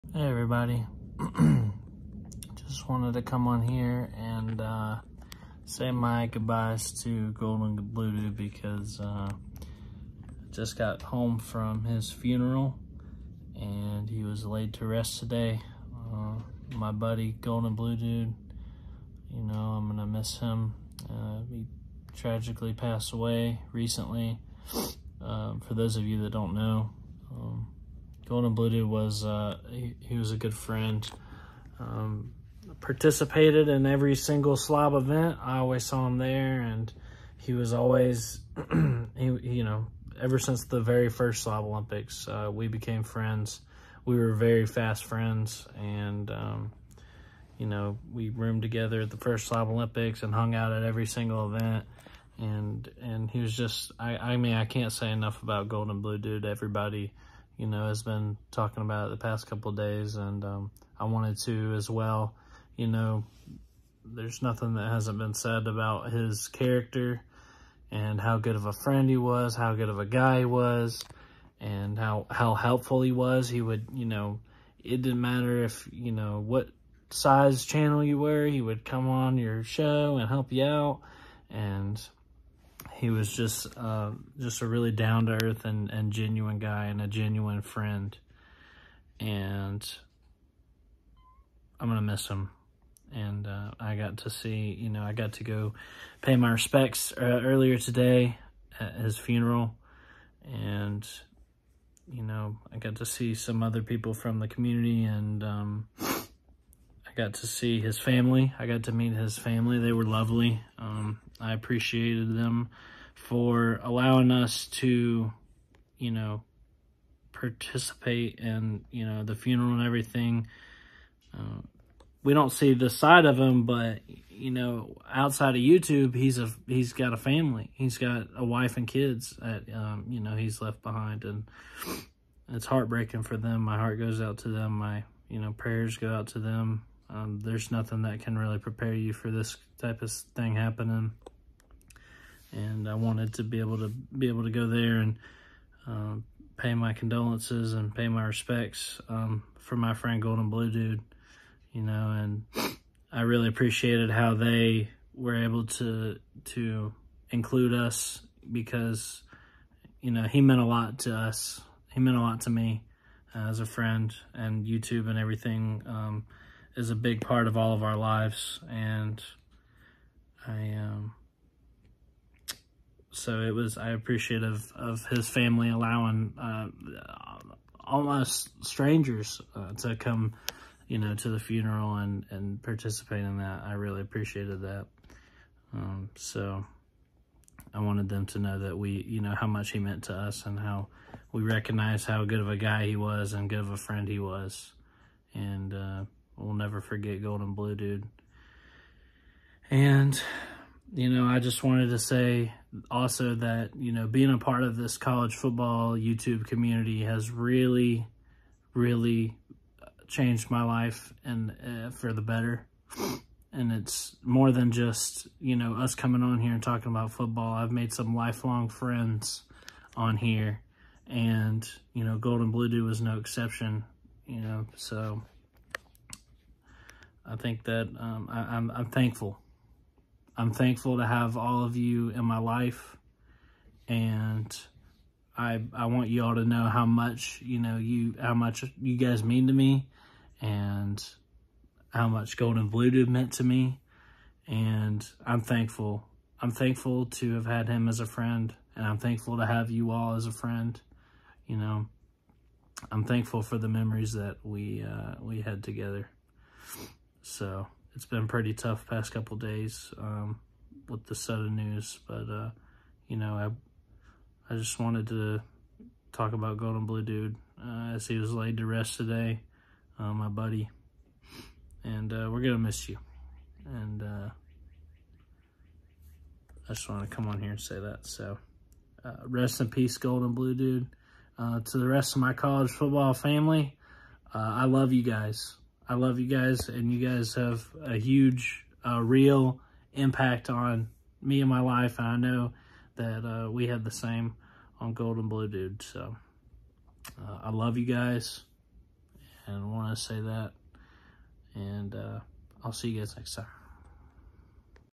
Rest In Peace Gold And Blue Dude
I encourage everybody to keep donating even though the goal was met, as that original goal would only cover the costs of the funeral itself, not other expenses like the casket. Let’s do our part to make sure that there is no financial burden on the family as well as the emotional one that they are already dealing with.